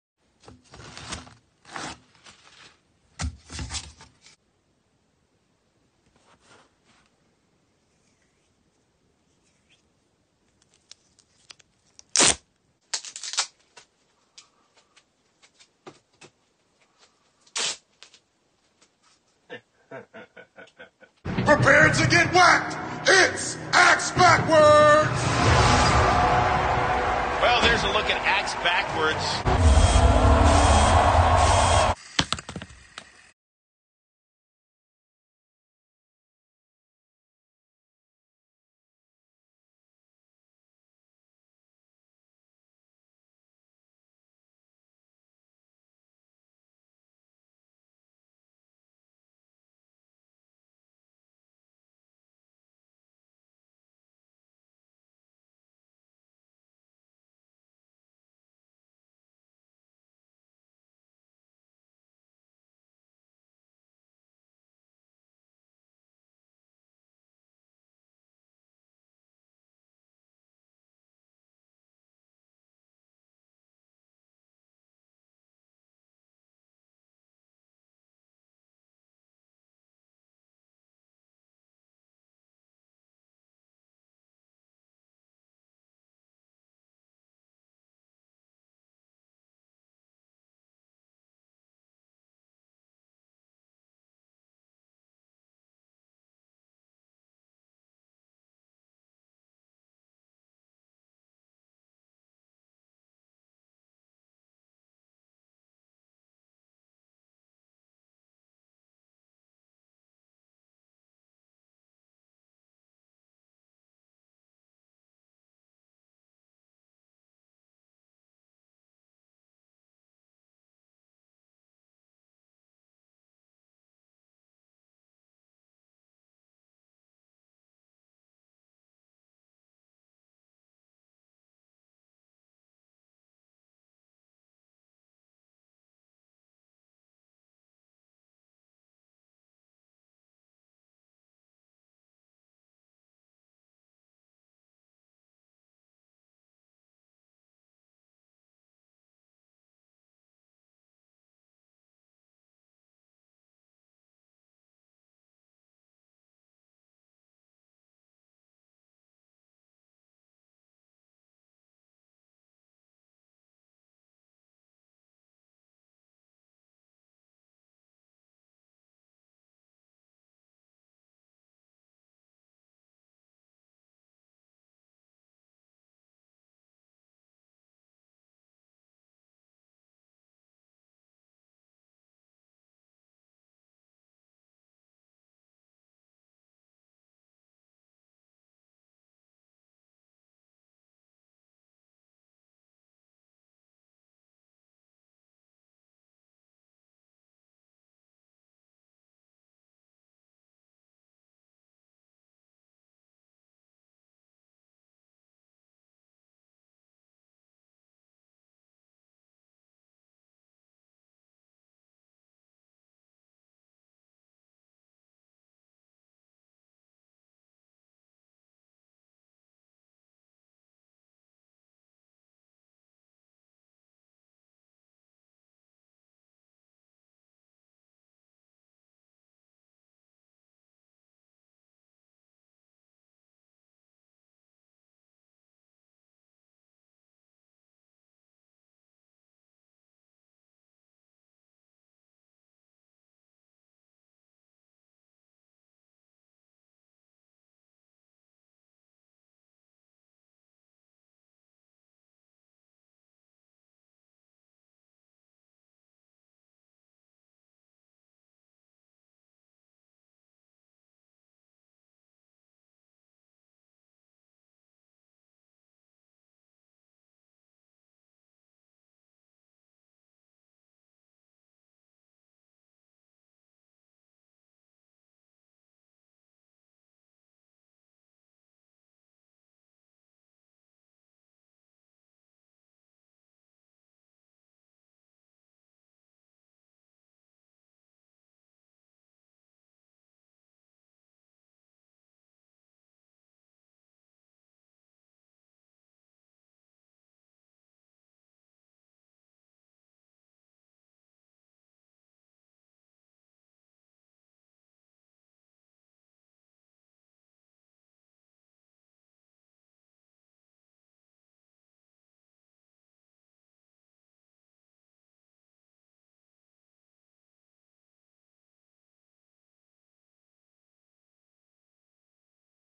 How To Build: Axe Backwards
In this video I'll show you how to build your very own Axe Backwards. It's very rare that I will do How to builds so give me a chance to get into it!

Yes, this is an April Fools I came up with a few weeks ago. I find it funny so I suppose that's all that matters really. i do however plan on building Axe Backwards properly as I really do like this machine and I can't wait to see the finished season 4 bot on Battlebots later this year! See you all soon!